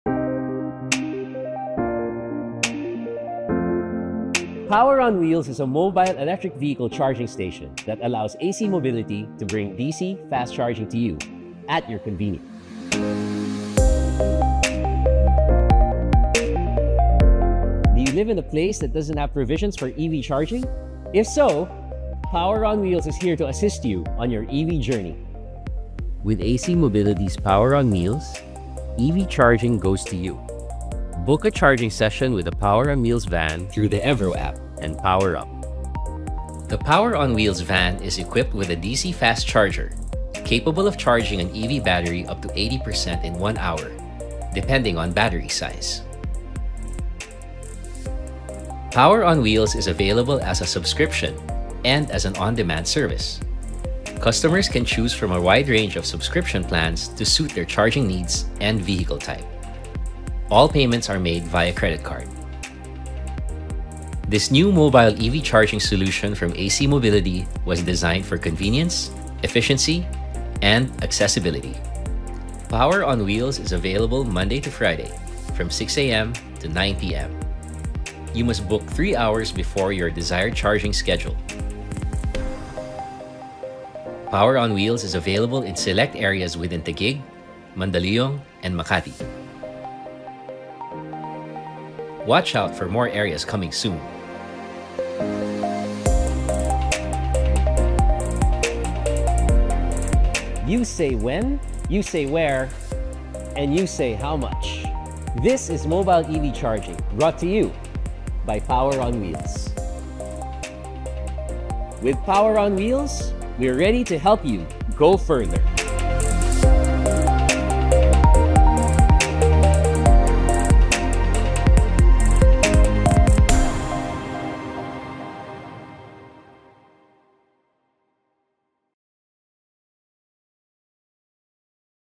What is Power-on-Wheels? | Power-on-Wheels PH
🚙⚡ Introducing Power-on-Wheels — the Philippine’s first Mobile EV Charging Station (MEVCS) by ACMobility!

No charging stations nearby? Power-on-Wheels brings DC charging straight to you!

📍Available in select areas of Makati, Mandaluyong, and Taguig.​

How to book:
1. On the Evro Station page, tap on the ACMobility Membership button on the upper right corner.
2. Enter car details and choose desired plan.
3. Register and pay via credit card.
🔌 DC Charger | CCS2 Port | Minimum 15kWh​